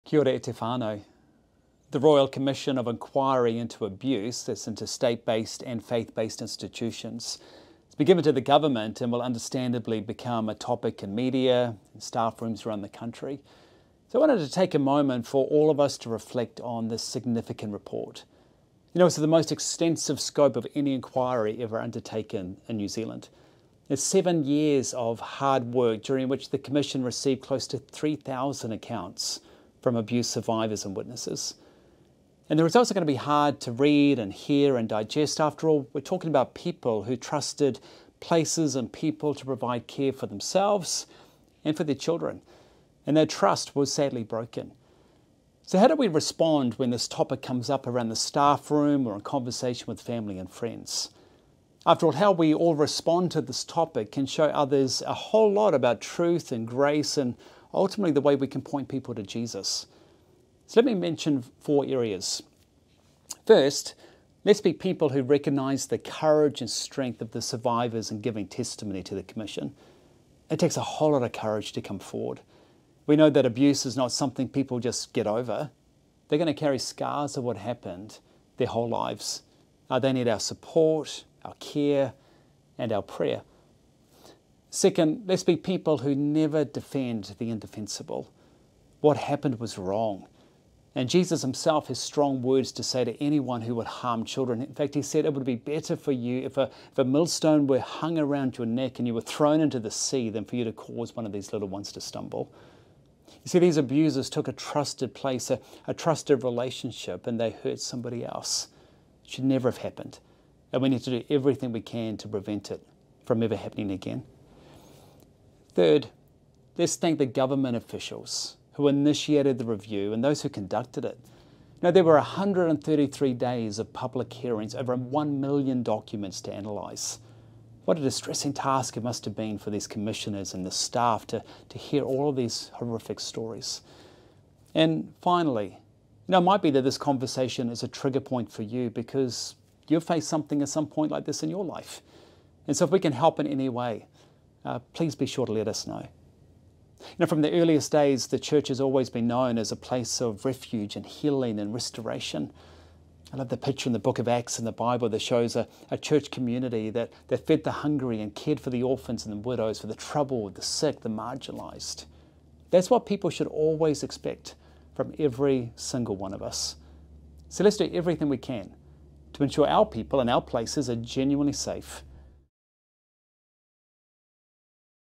A Message on the Abuse in Care Royal Commission | Jonathan Dove | Auckland Church Network Chair
Jonathan Dove, Chair of Auckland Church Network, provides some reflections on the forthcoming final report of the Royal Commission Inquiry into Abuse in Care